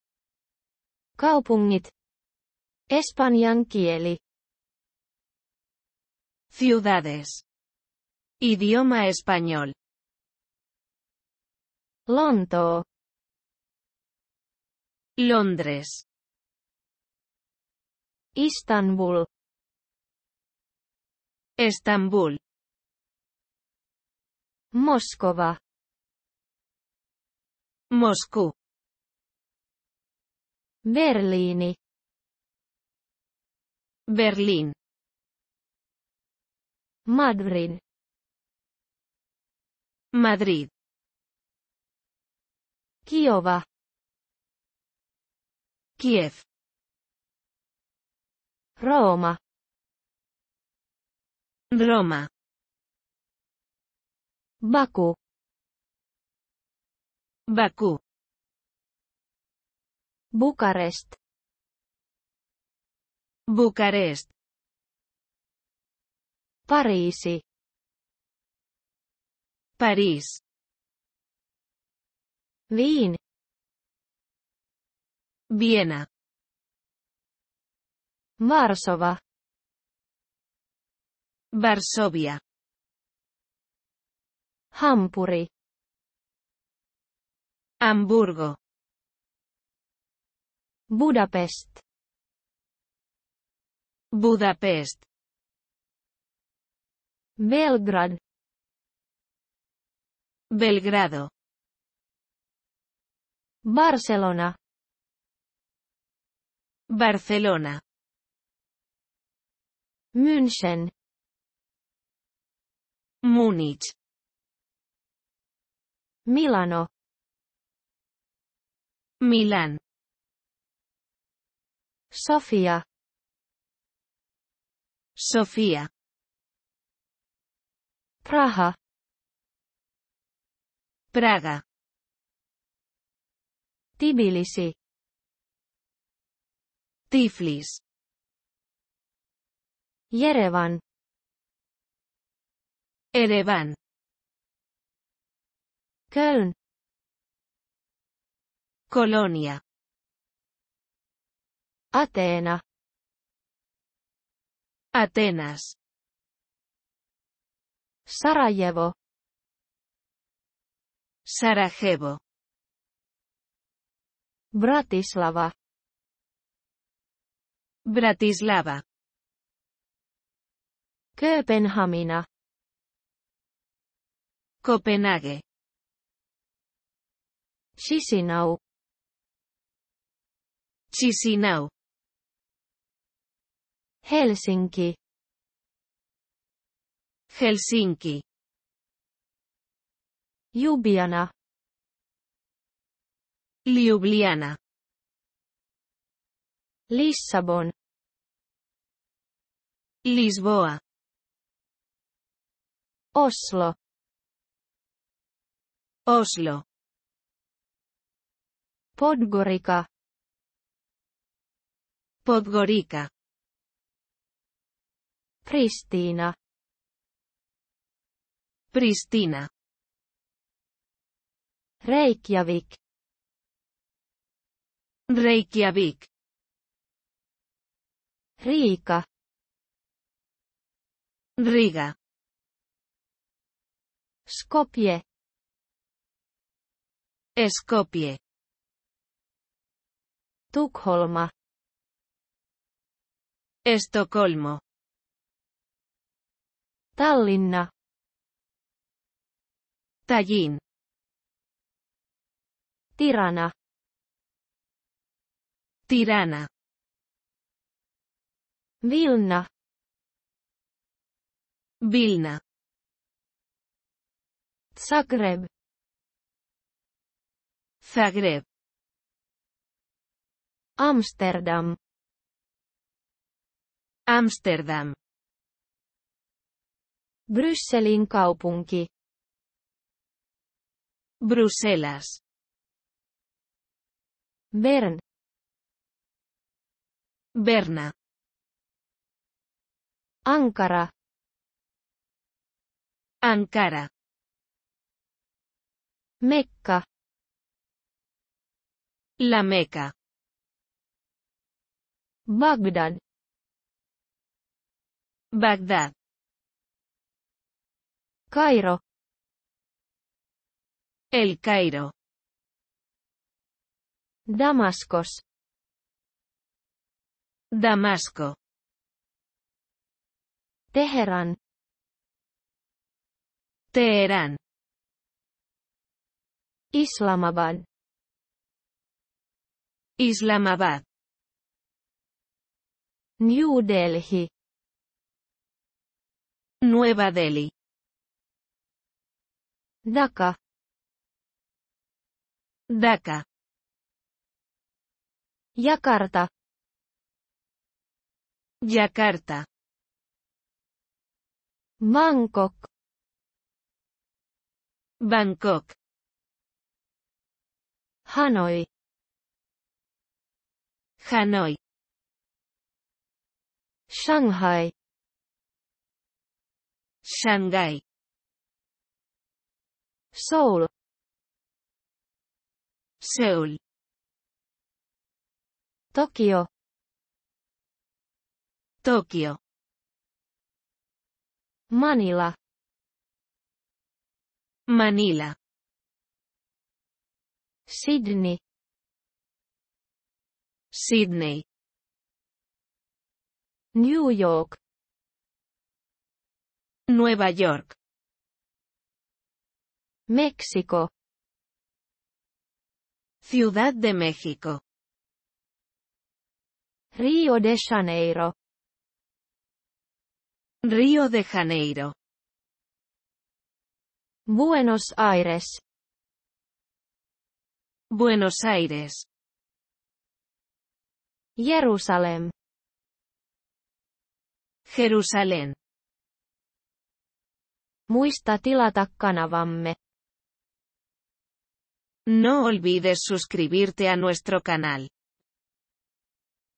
Kaupungit (Espanjan kieli) (fi-es)
Fone TV
fi-es-30
Ciudades (Idioma español)
Lontoo - Londres
Istanbul - Estambul
Moskova - Moscú
Berliini - Berlín
Madrid - Madrid
Kiova - Kiev
Rooma - Roma
Baku - Bakú
Bukarest - Bucarest
Pariisi - París
Wien - Viena
Varsova - Varsovia
Hampuri - Hamburgo
Budapest - Budapest
Belgrad - Belgrado
Barcelona - Barcelona
München - Múnich
Milano - Milán
Sofia - Sofía
Praha - Praga
Tbilisi - Tiflis
Jerevan - Ereván
Köln - Colonia
Ateena - Atenas
Sarajevo - Sarajevo
Bratislava - Bratislava
Kööpenhamina - Copenhague
Chișinău - Chisináu
Helsinki - Helsinki
Ljubljana - Liubliana
Lissabon - Lisboa
Oslo - Oslo
Podgorica - Podgorica
Pristina - Pristina
Reykjavík - Reikiavik
Riika - Riga
Skopje - Skopie
Tukholma - Estocolmo
Tallinna - Tallin
Tirana - Tirana
Vilna - Vilna
Zagreb - Zagreb
Amsterdam - Ámsterdam
Brysselin kaupunki - Bruselas
Bern - Berna
Ankara - Ankara
Mekka - La Meca
Bagdad - Bagdad
Kairo - El Cairo
Damaskos - Damasco
Teheran - Teherán
Islamabad - Islamabad
New Delhi - Nueva Delhi
Dhaka - Daca
Jakarta - Yakarta
Bangkok - Bangkok
Hanoi - Hanói
Shanghai - Shanghái
Soul - Seúl
Tokio - Tokio
Manila - Manila
Sydney - Sídney
New York - Nueva York
México - Ciudad de México
Rio de Janeiro - Río de Janeiro
Buenos Aires - Buenos Aires
Jerusalem - Jerusalén